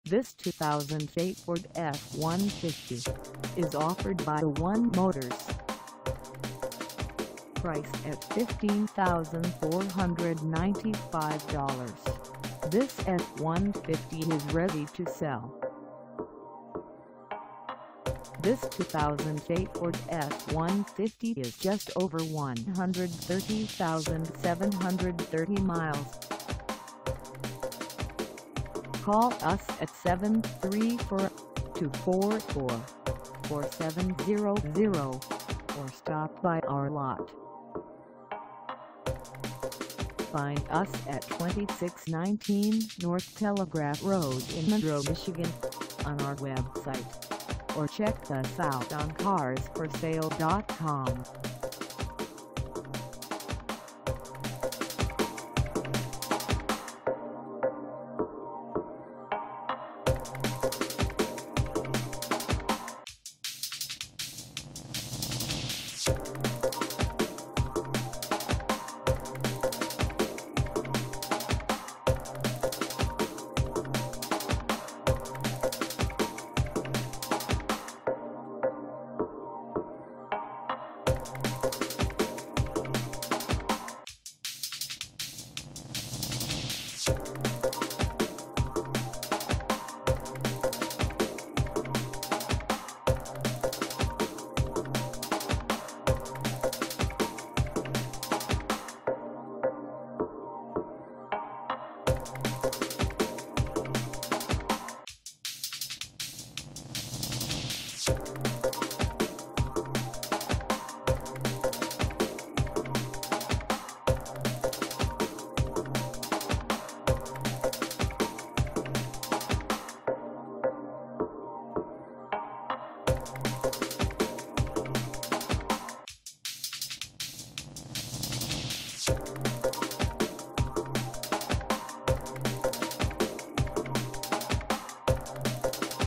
2008 Ford F150 Lariat SuperCrew Short Box 4WD for sale in Mo
This 2008 Ford F150 Lariat SuperCrew Short Box 4WD is for sale in Monroe, MI 48162 at A 1 Motors.
Engine: 8-Cylinder V8, 5.4L; FFV
Transmission: 4 Speed Automatic
Color: Blue
Mileage: 130730
Contact A 1 Motors 2619 N Telegraph Rd Monroe, MI 48162 to test drive this 2008 Ford F150 Lariat SuperCrew Short Box 4WD today.